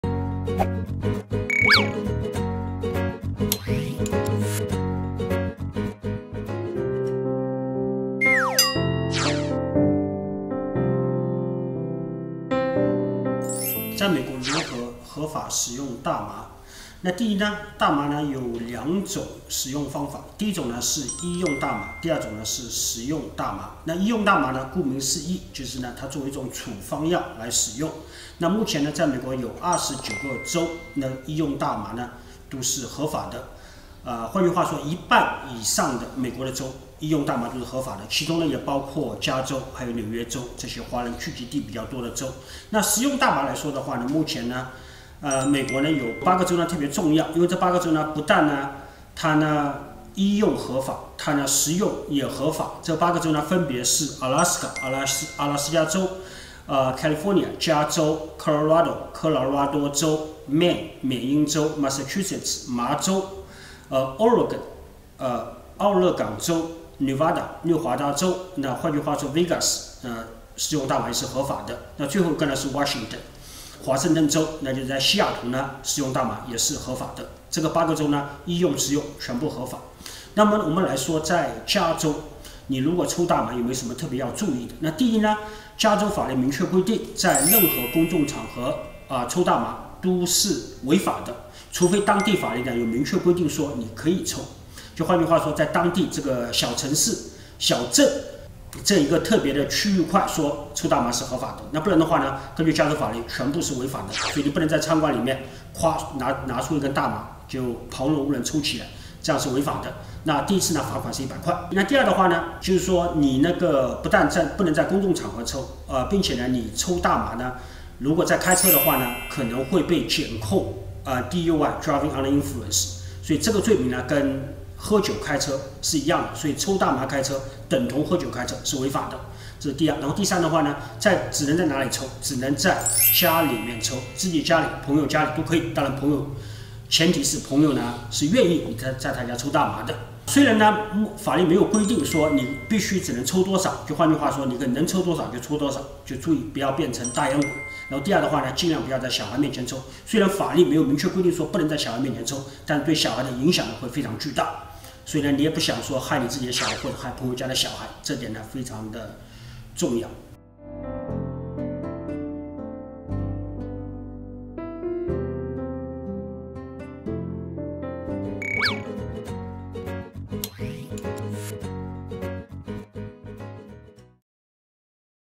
在美国如何合法使用大麻？|法律讲堂 Use Marijuana Legally
大麻在美国的很多州都已经合法化，但医用和食用大麻还是有很多的相关法律法规，那么，究竟两者如何区分？哪些州是被允许合法使用的？有什么注意事项？ 这期How视频告诉您在美国如何合法使用大麻。

合法使用大麻：

合法使用大麻的种类:医用大麻、食用大麻。

在美国有29个州可以合法使用医用大麻。

在美国有8个州医用和食用大麻都合法。

食用和医用大麻合法的州：
阿拉斯加州、加利福尼亚州、科罗拉多州、缅因州、马萨诸塞州、俄勒冈州、内华达州、华盛顿州。

在加州使用大麻注意事项：

注意事项一在任何公众区域食用大麻都是违法的
如当地城市有特别规定则可在工作场合吸食大麻。

注意事项二吸食大麻后开车会被检控
吸食大麻后开车会被监控DUI（ driving under influence），醉酒或受药物影响驾车。

注意事项三在朋友家吸食大麻最好获得朋友允许
可以在自己家里或朋友家里吸食大麻，但前提是获得朋友允许。

加州法律没有规定吸食大麻的数量，但律师建议吸食大麻不要过量，不要在未成年人面前吸食大麻。